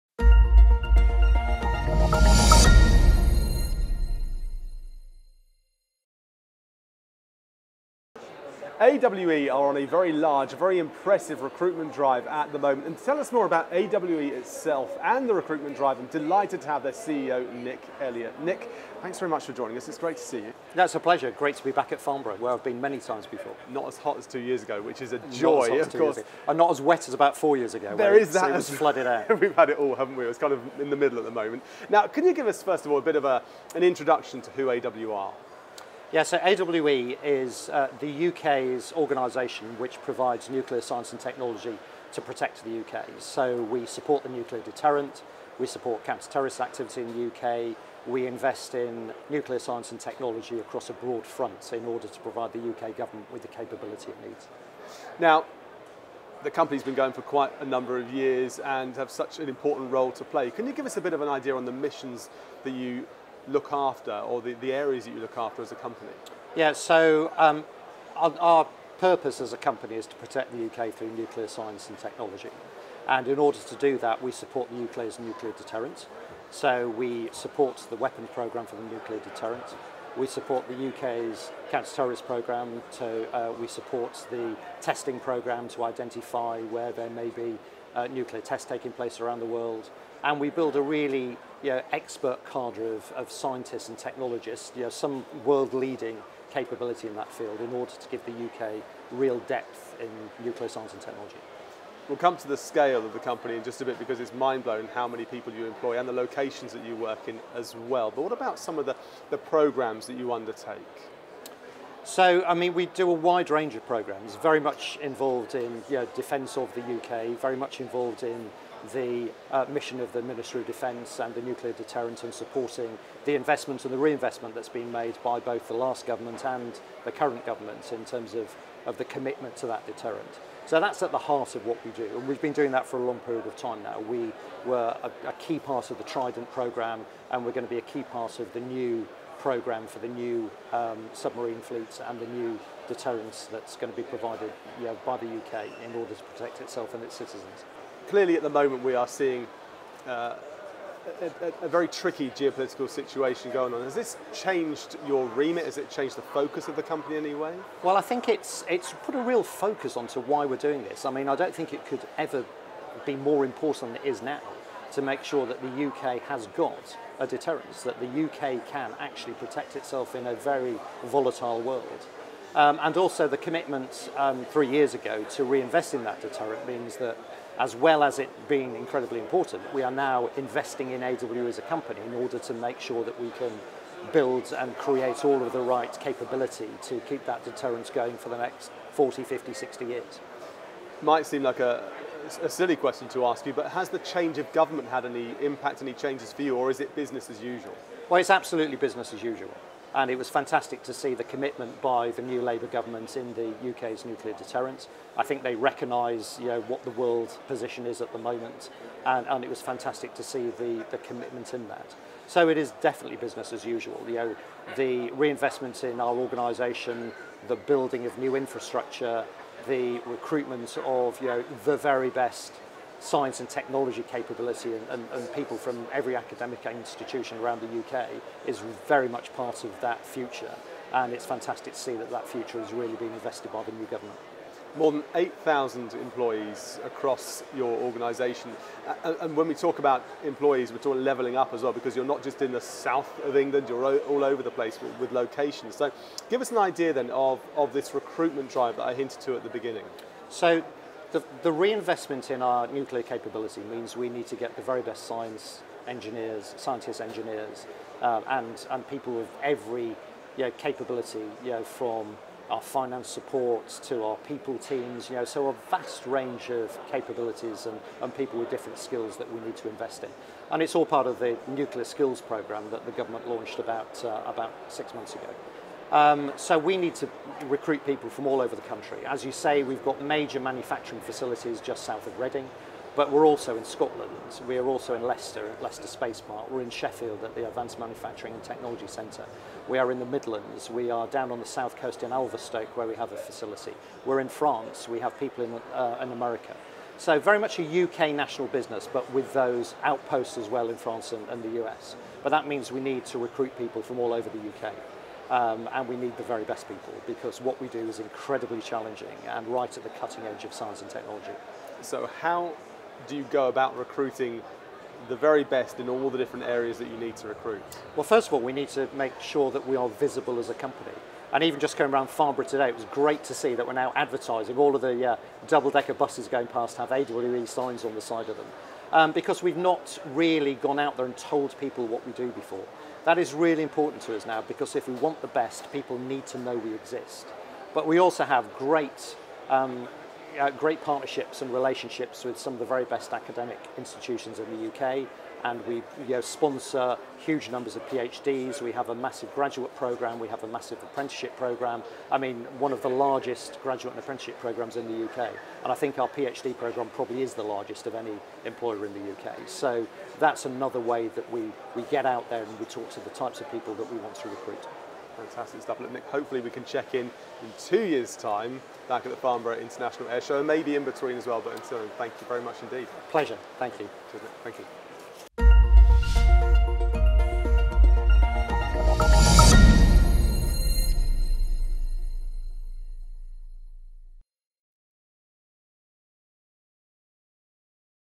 AWE nurturing new talent in UK-wide campaign
AWE – the group responsible for supporting the UK government’s nuclear defence strategy for more than 70 years – has highlighted the importance of recruiting new talent to the sector, with its mission possibly more important than ever given current geopolitical tension.